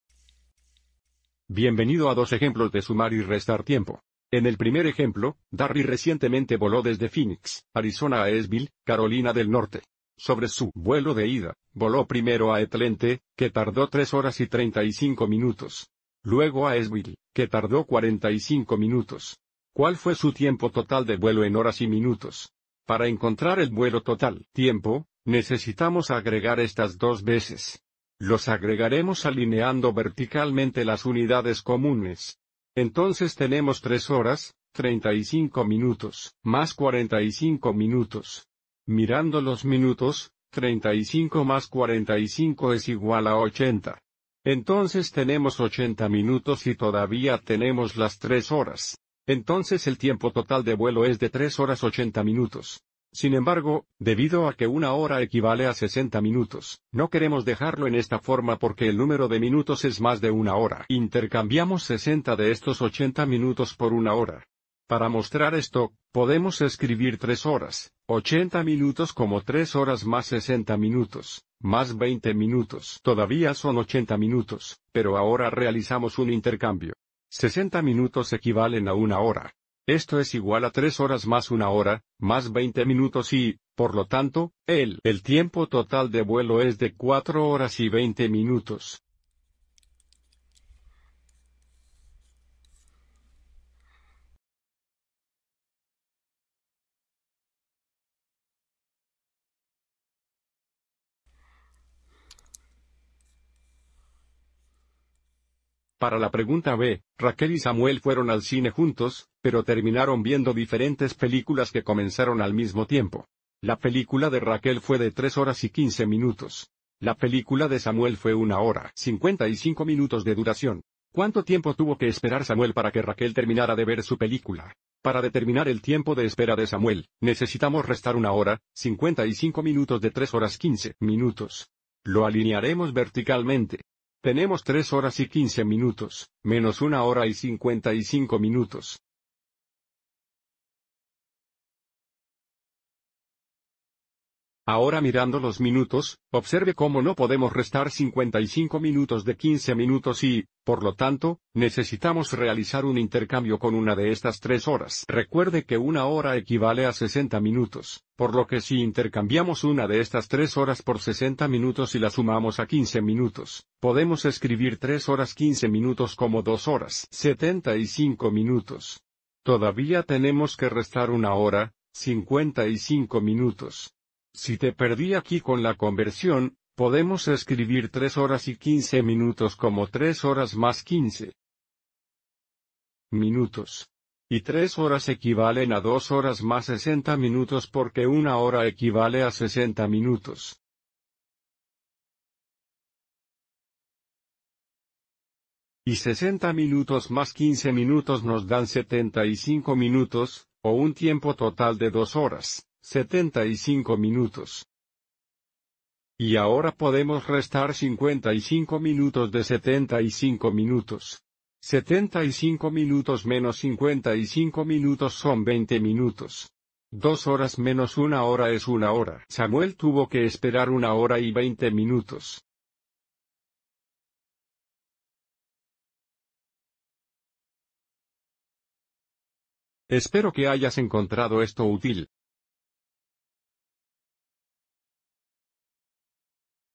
Agregar y restar tiempo (horas y minutos)
Este video explica cómo sumar y restar el tiempo dado en horas y minutos.